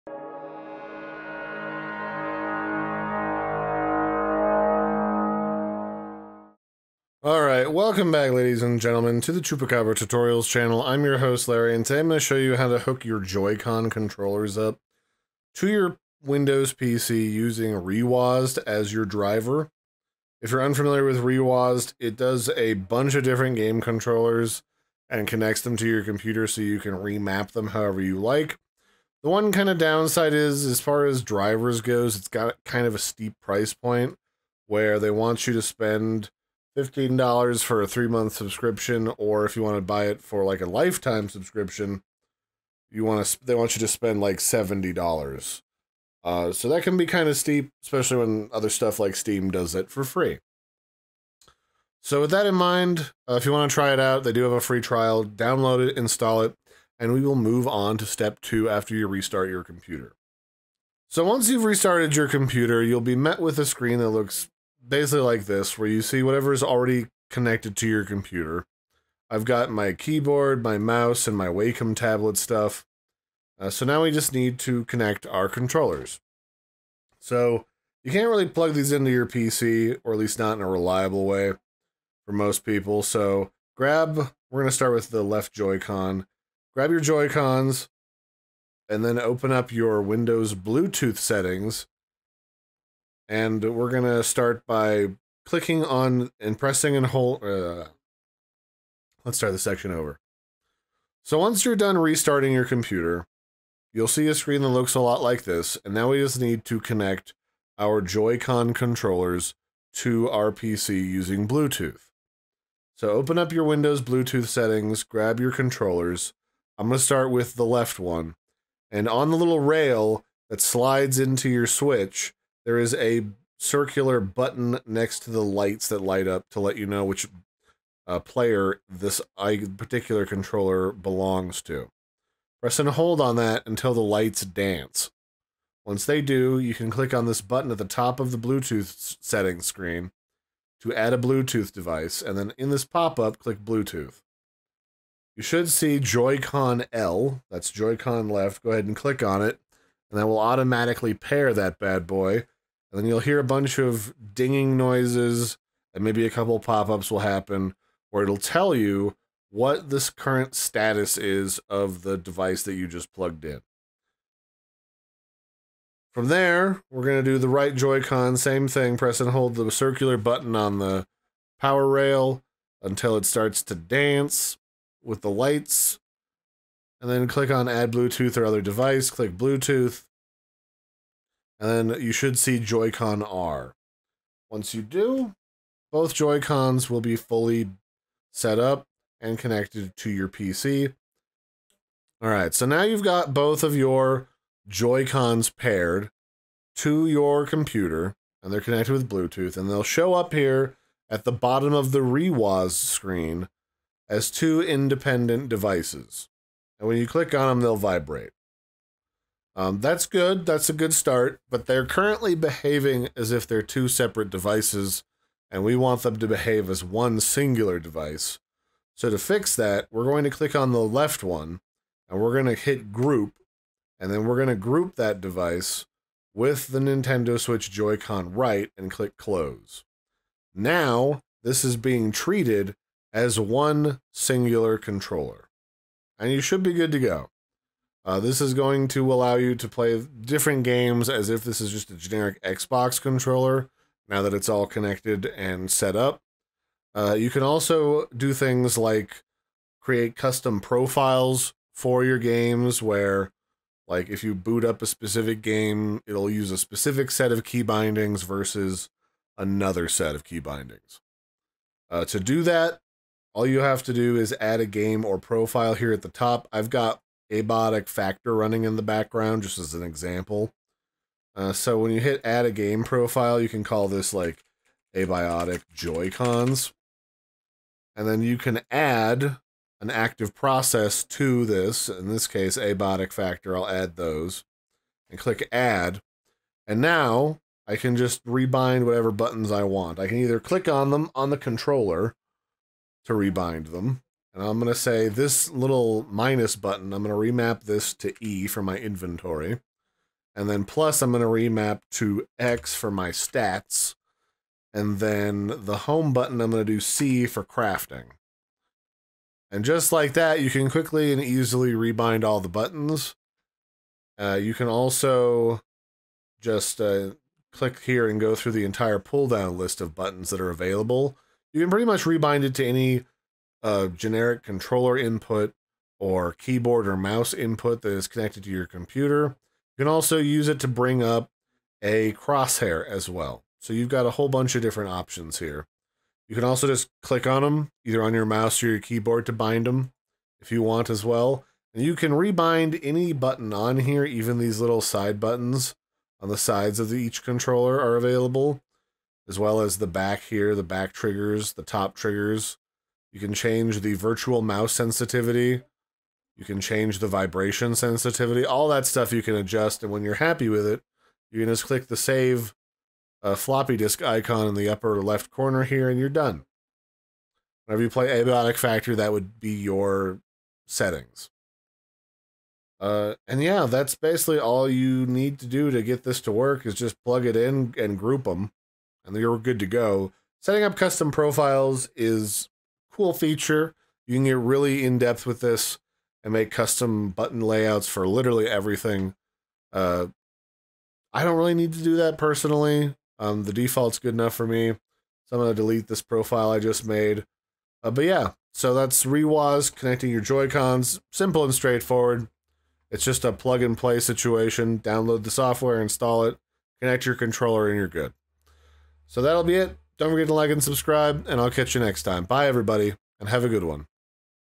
How to Use Switch Joy-Cons on PC with reWASD (Bluetooth Setup + Custom Keybinds) 🎮 2025 Tutorial
Looking to use your Nintendo Switch Joy-Cons on PC for gaming or emulation? 🎮 In this updated 2025 tutorial, I’ll walk you through how to connect Joy-Cons to your PC via Bluetooth and set them up in reWASD to create fully custom keybinds and presets.

Whether you want to use both Joy-Cons as a full controller or separately for multiplayer games, this guide shows you everything you need to know to get started with zero drift and full customization.

🔧 What You’ll Learn in This Video:
✅ How to connect Joy-Cons to PC via Bluetooth
✅ How to use reWASD to recognize both Joy-Cons
✅ How to sync Joy-Cons as a single controller or two separate ones
✅ Creating and applying custom keybind presets
✅ Rebinding buttons for Steam, emulators, and other PC games
✅ Saving profiles and swapping layouts on the fly

🧠 System Requirements:
Bluetooth-enabled PC
Nintendo Switch Joy-Cons
reWASD installed (free for basic remapping, paid for advanced features)